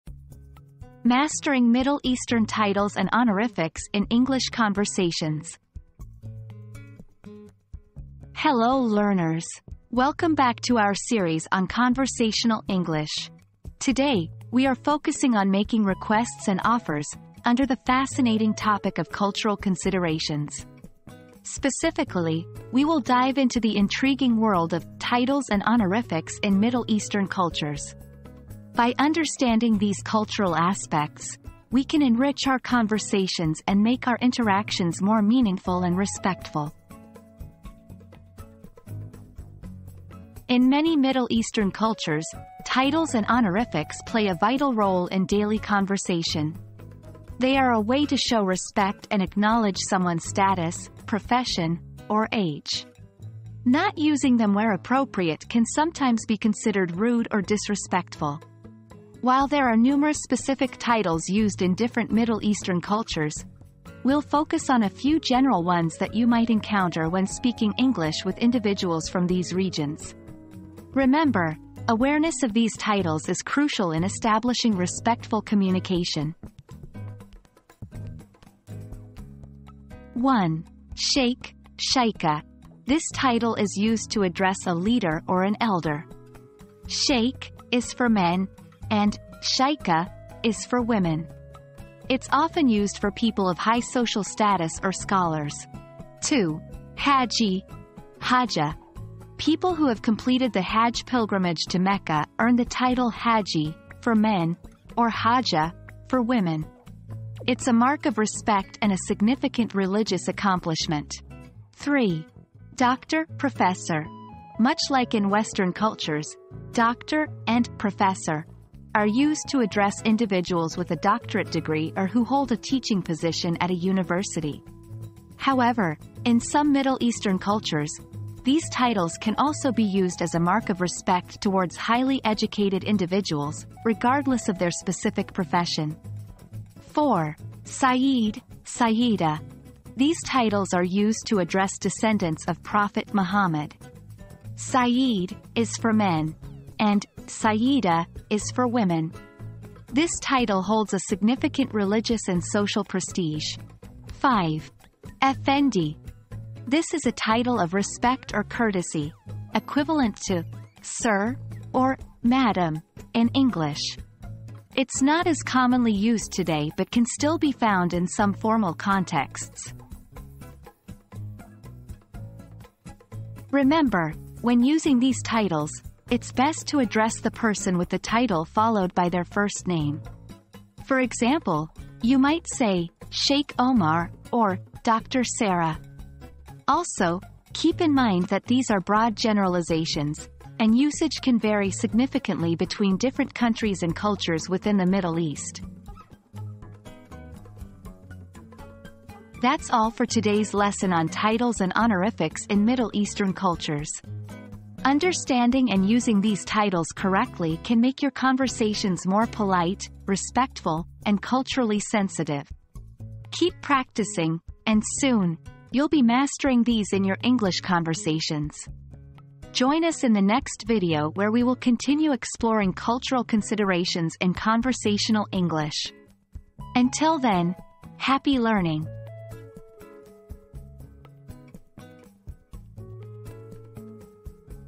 Mastering Middle Eastern Titles and Honorifics in English Conversations
Mastering Middle Eastern Titles and Honorifics in English Conversations • Learn about the importance of titles and honorifics in Middle Eastern cultures and how to use them respectfully in English conversations.

00:00 • Introduction - Mastering Middle Eastern Titles and Honorifics in English Conversations
00:39 • The Importance of Titles and Honorifics
01:19 • Common Titles and Honorifics
02:52 • Usage in Conversation